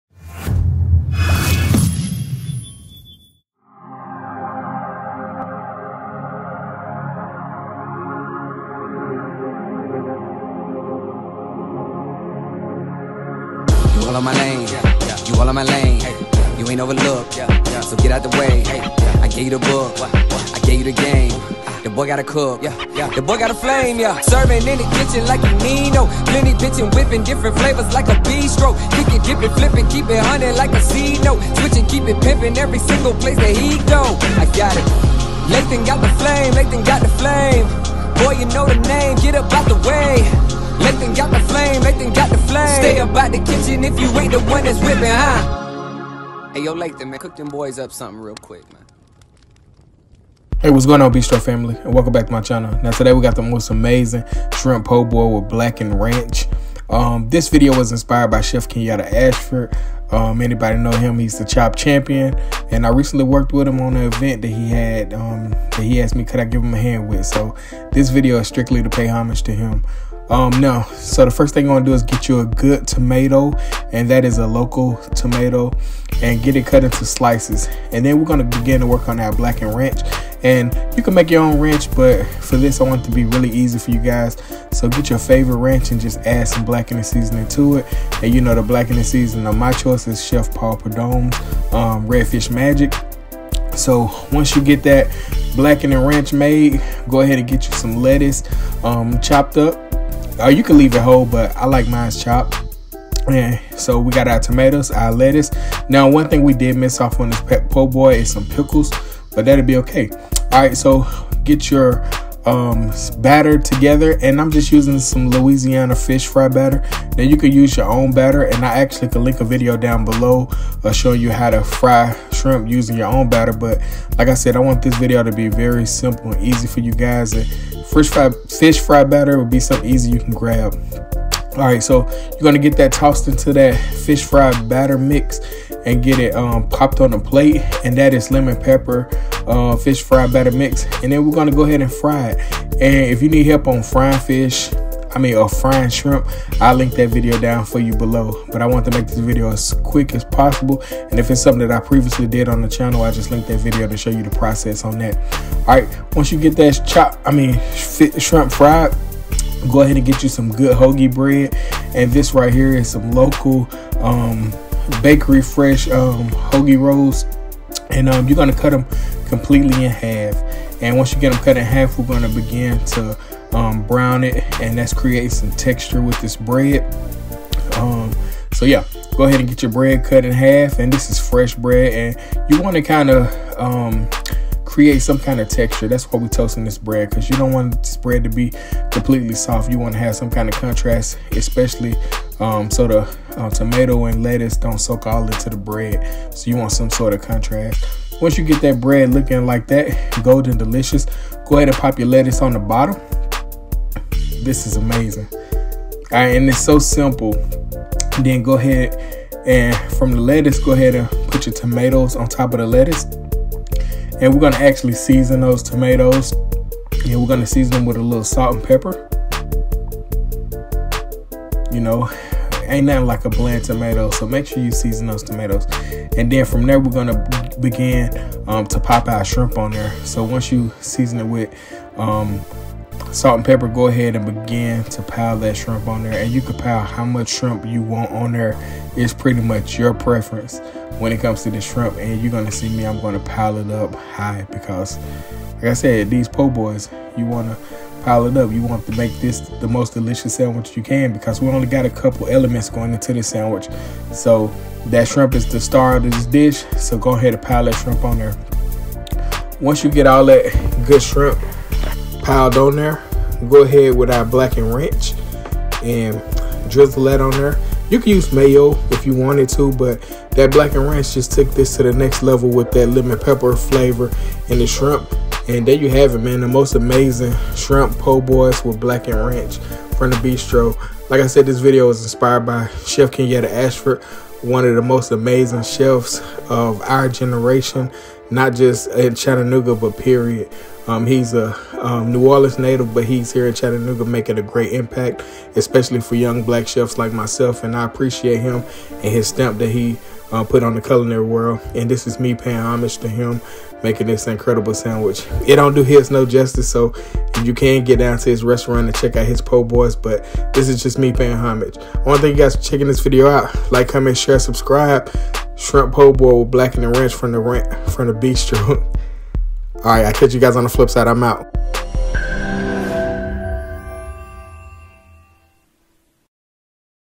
Shrimp Po'Boy With Blackened Ranch
This recipe for my Shrimp Po’boy with blackened Ranch is an Easy How To video it’s a simple recipe but for sure a game changer and a crowd pleaser…… It pays homage to the my favorite Chef Kenyatta Ashford and it’s simply just a Great shrimp recipe to share with your friends and family for years to come….Hope you all enjoy this recipe…. For more recipes like this

Intro song was written and performed performed by J.Flo

Background Instrumental (Bistro Music)
Produced by Kee Ford